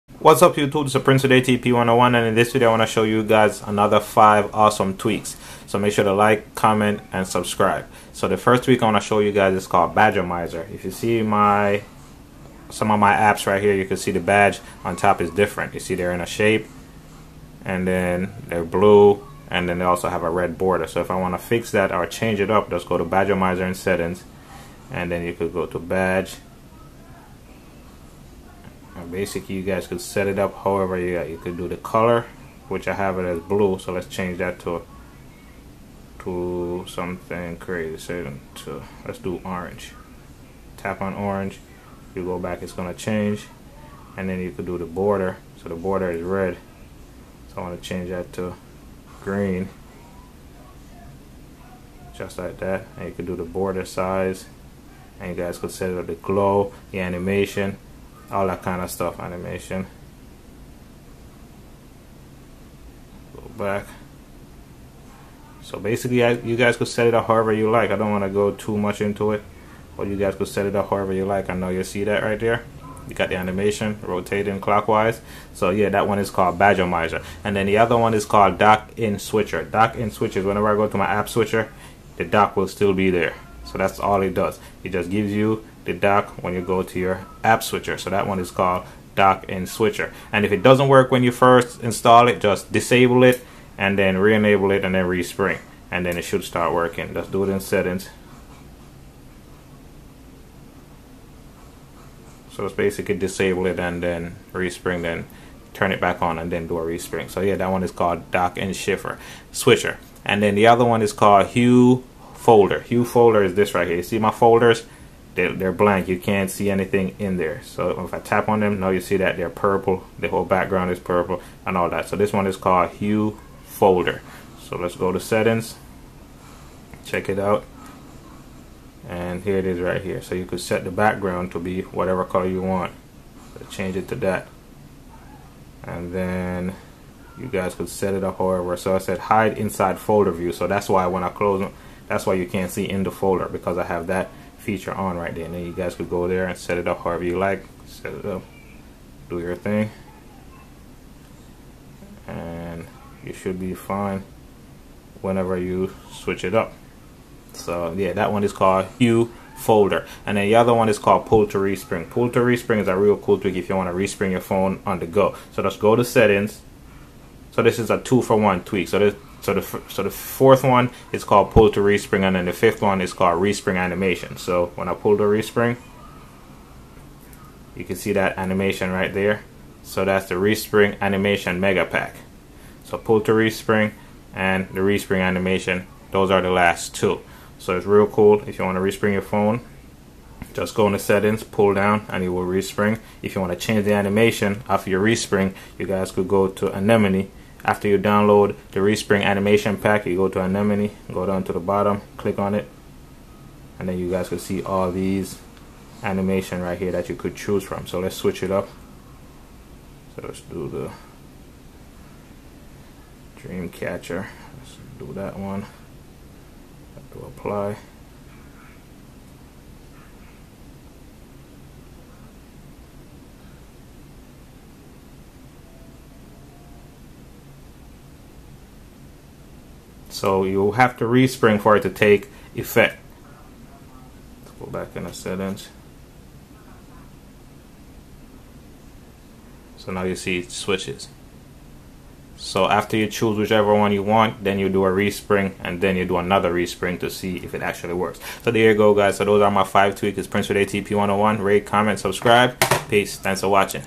Top 5 Awesome Tweaks iOS 11.2 - 11.4
Badgomizer
DockInSwitcher
HueFolder
PullToRespring
Respring Animation Mega Pack